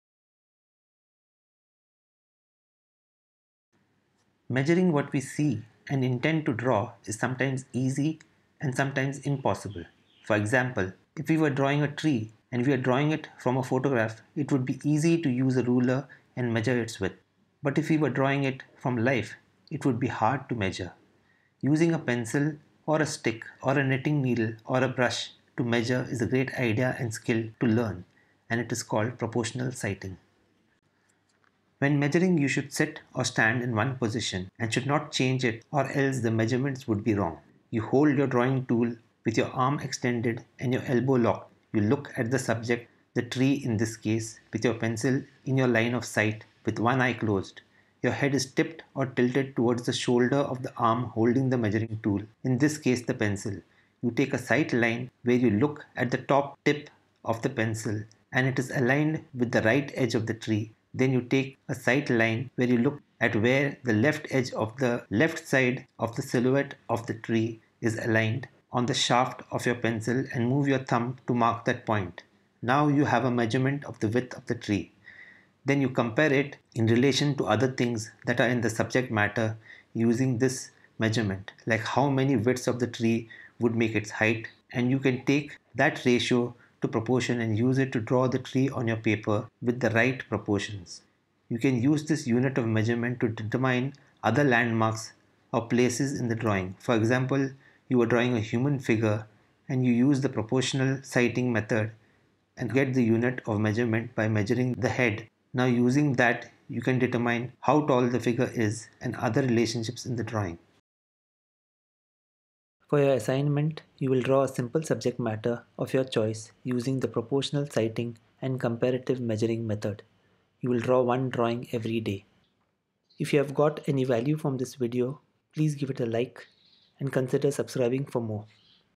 Fundamentals Of Drawing For Beginners -Week 25 - Proportional sighting & Comparative measuring
In this video i talk about measuring and proportions and the Proportional sighting and comparative measuring technique.